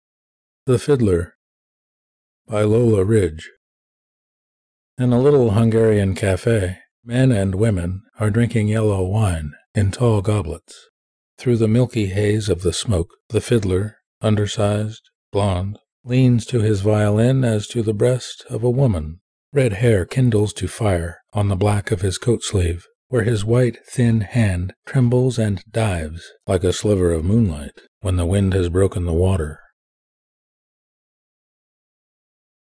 "The Fiddler," by Lola Ridge
Poem, in the public domain. Ridge (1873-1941) was an Irish-American and is considered to have been among the Imagists.  She was also an Anarchist. Recording is mine. Images from wikimedia commons.